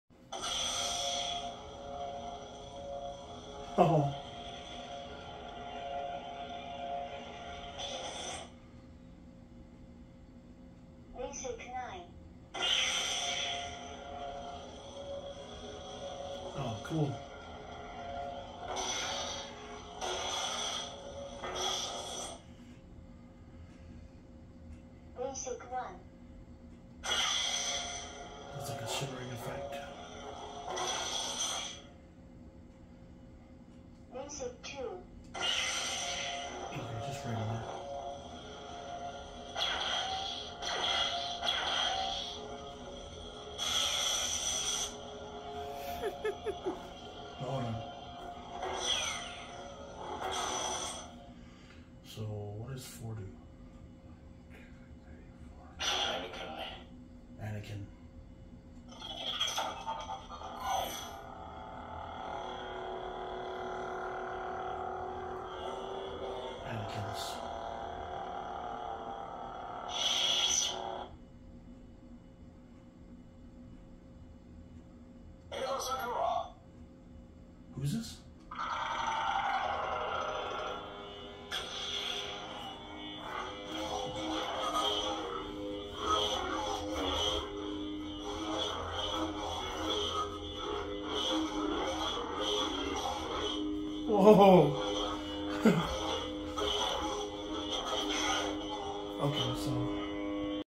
Mika's new Lightsaber 2023
Mika testing out all the setting on the new Mandalorian light saber. He is liking all the new fonts/music/voices and 11 different colors with this saber.

Thanks for visiting my channel and watching this video. Please visit us again and Have a blessed day!